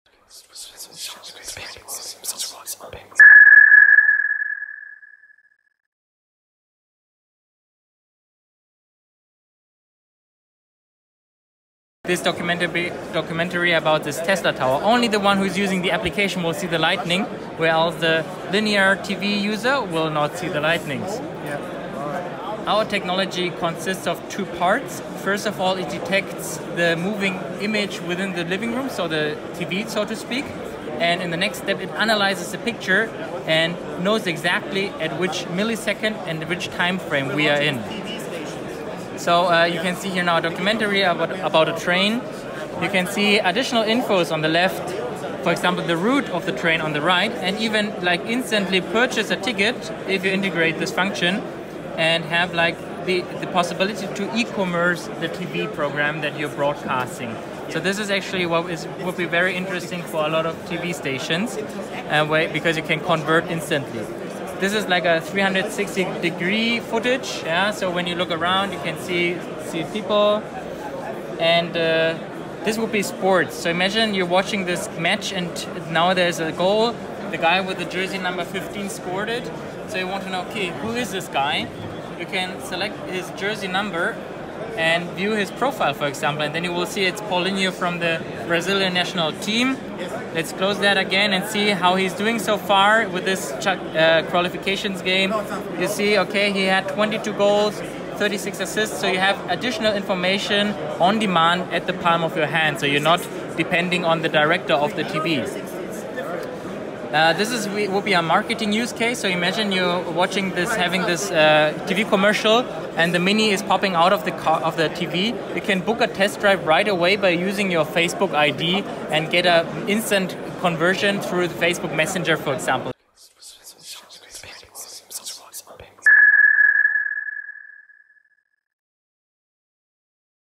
Future of Televison with Augmented Reality
This is television using Augmented Reality in the near future. During sometime in November 2017 a German TV station will experiment live with AR and TV content. Call it Mixed Reality or even Extended Reality. This showcase by Augmentv was filmed at the Augmented World Expo in Munich 2017 by MediaFusion.